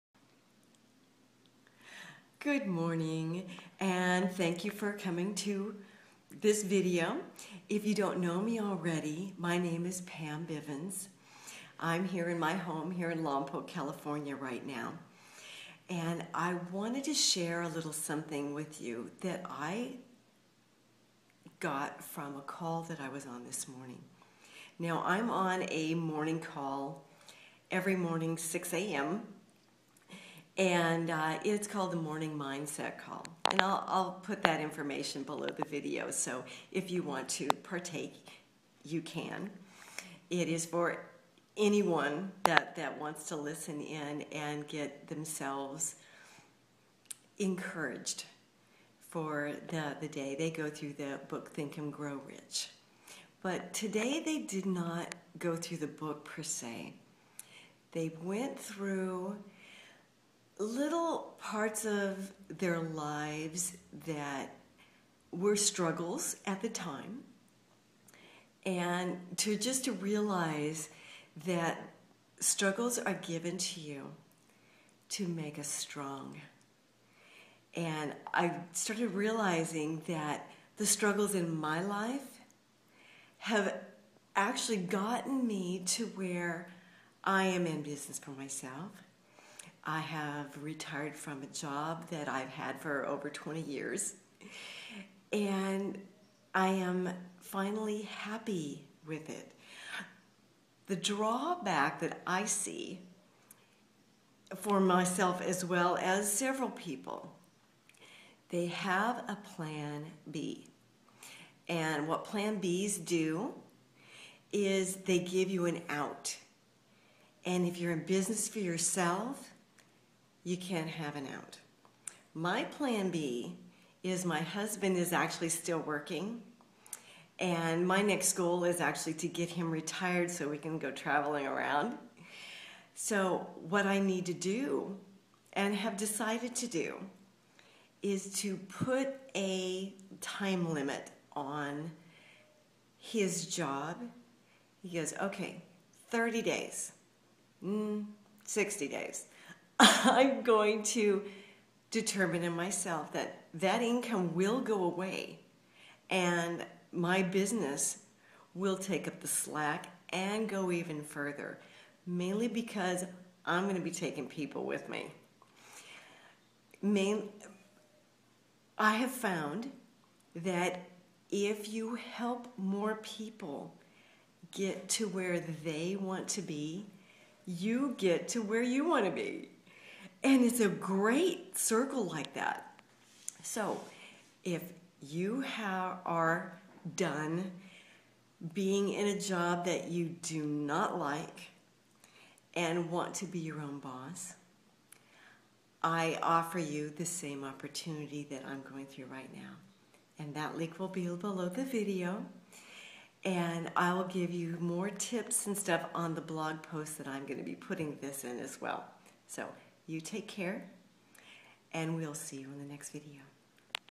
Do you have a Plan B - Day 16
Having a Plan B has the possibility of holding you back when it comes to making Plan A work and getting what you want out of it.  What I mean by that is, when you have a plan to fall back on, you tend to not do everything possible to make your business work.  Adjusting along the way is a good thing, but having a plan B allows you to give up just before you make that all important breakthrough!!

Looking specifically at a business as a whole, you want to do everything you can to make it work.

A plan B is giving you an out, or a way to cop out or quit,  so you will not work at your business as hard as you need to to make it work!

Think of your past struggles and how they've made you so strong and more persistent in order to get you to reach the goals that you want your business or your life to get to.  Realize that any new experience has it's own set of struggles and learning curves.  Give yourself permission to take the time needed to learn what you need to make your goals come true.  You need an unwavering determination and no Plan B that will allow you to quit.

This is what I have done with my business - No Plan B!!

I will build my business and others up to where we all are finally living the kind of lives that we have always dreamed of!

Find out more about how I was able to retire after just 4 months